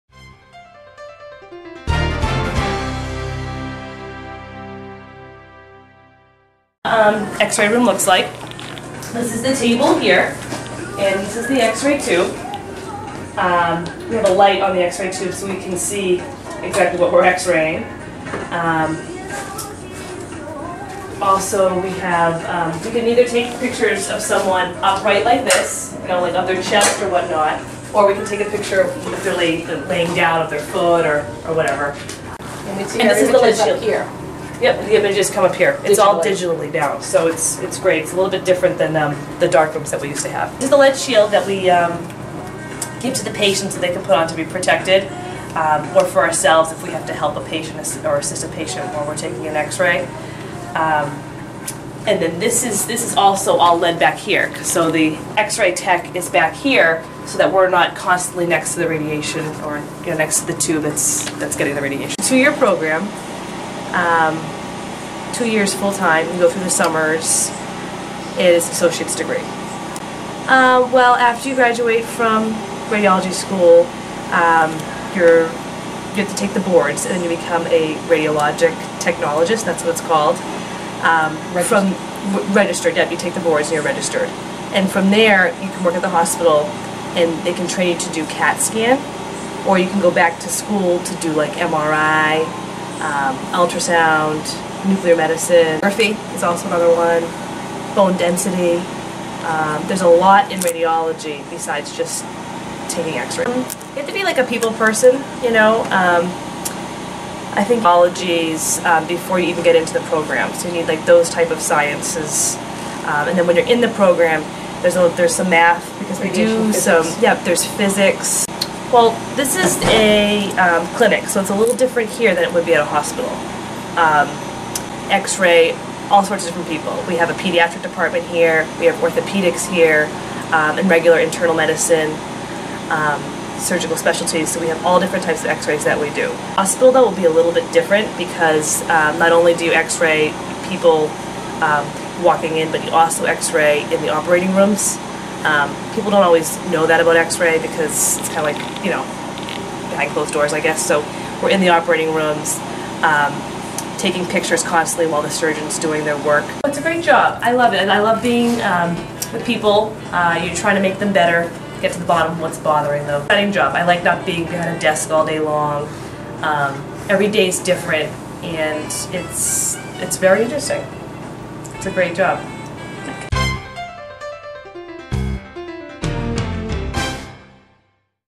Radiologic Technician/Technologist
Typical Day, Tasks, Education and Work Environment for Radiologic Technicians and Technologists
If you like this video vote for it on the US DOL Career challenge before 12/31/2010 at :